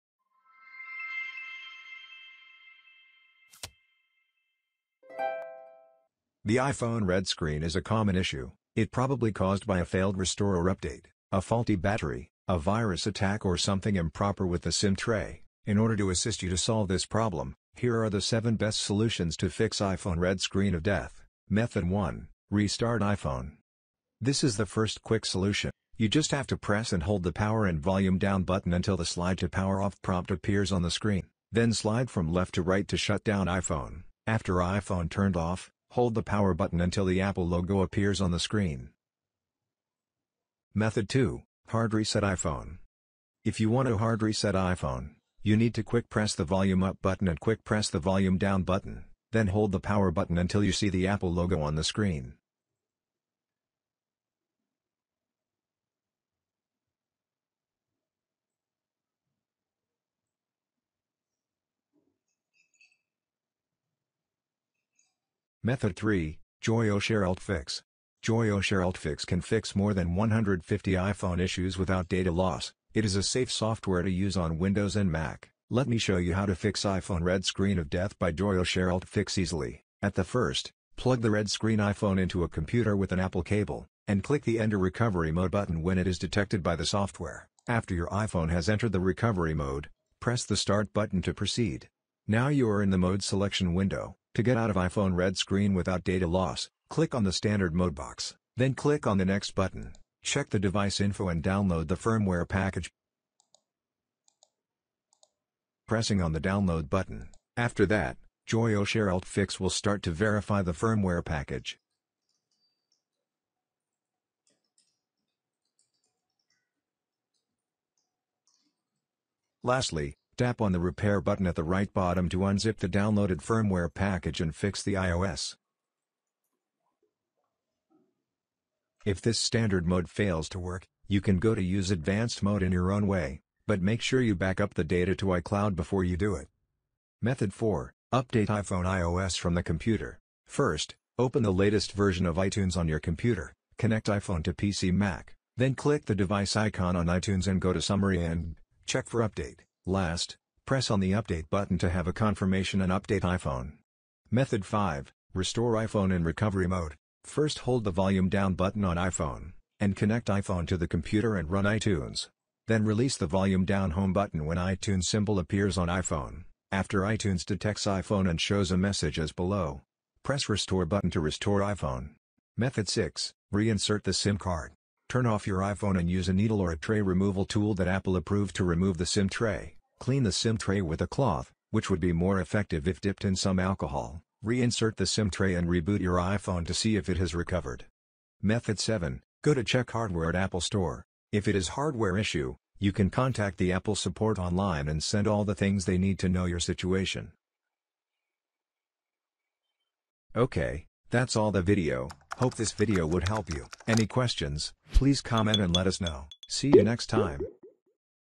How to Fix iPhone Red Screen of Death without Data Loss | Joyoshare UltFix
Use Joyoshare UltFix to fix iPhone red screen of death without losing your data
Learn how to fix iPhone red screen in this video. If you are interested in this topic, you can go to read the full article

The iPhone red screen is a common iPhone glitch that happens frequently. What to do when your iPhone is stuck on the red screen? The probable causes include a failed restore or update, a faulty battery, a virus attack, or something improper with the SIM tray. In order to assist you to solve this problem, here are the 7 best solutions that targeted different issue causes.

In some cases, system error will result in iPhone red screen. The easiest solution is to use a third-party iOS repairing tool like Joyoshare UltFix. It is a professional iOS system recovery tool that can fix more than 150 iOS system issues without data loss.

Method Covered:
0:00 Introduction
0:08 What Causes Lines on iPhone Screen
0:20 Method 1: Restart iPhone
0:40 Method 2: Hard Reset iPhone
01:05 Method 3: Joyoshare UltFix (Without data loss)
02:17 Method 4: Update iPhone iOS from the Computer
02:34 Method 5: Restore iPhone in Recovery Mode
02:52 Method 6: Reinsert the SIM Card
03:08 Method 7: Go to Check Hardware at Apple Store